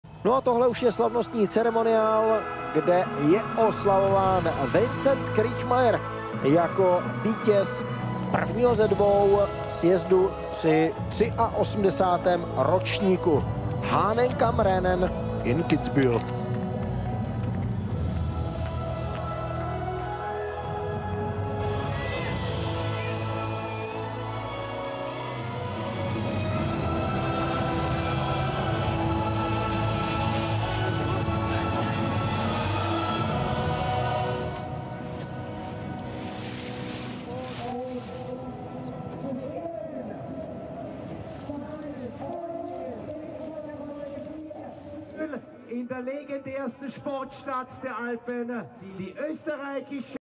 Men's Downhill - Award Ceremony - Kitzbühel AUT - 2023
1. Vincent KRIECHMAYR  2. Florian SCHIEDER  3. Niels HINTERMANN   I   FIS Alpine Ski World Cup - Full HD